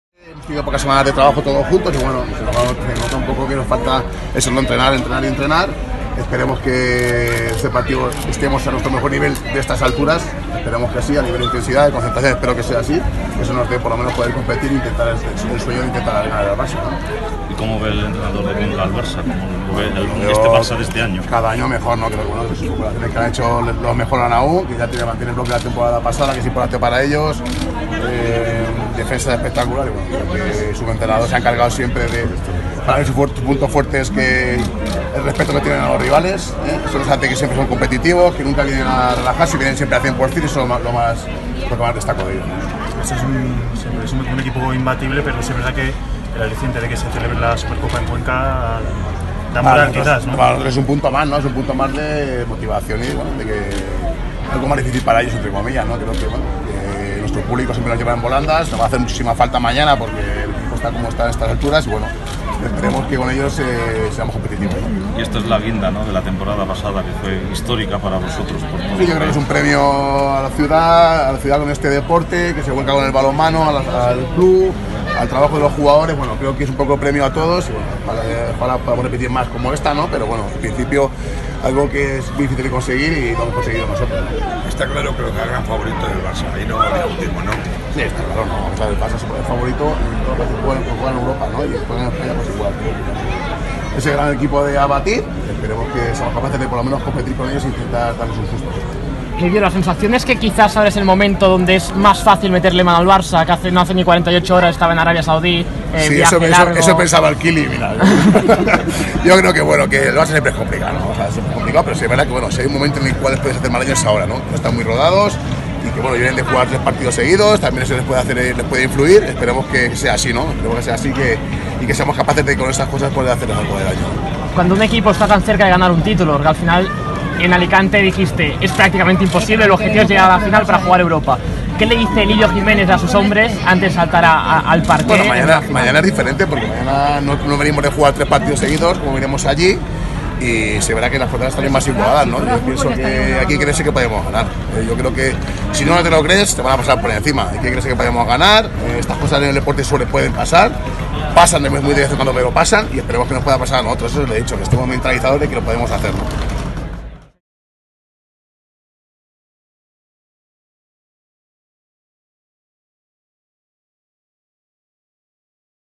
Supercopa ASOBAL 2019 | Lidio Jiménez analiza la previa del encuentro
El entrenador del Liberbank Cuenca,  Lidio Jiménez analiza la previa al encuentro de la Supercopa ASOBAL 2019 que se disputa este miércoles en el pabellón municipal El Sargal de Cuenca.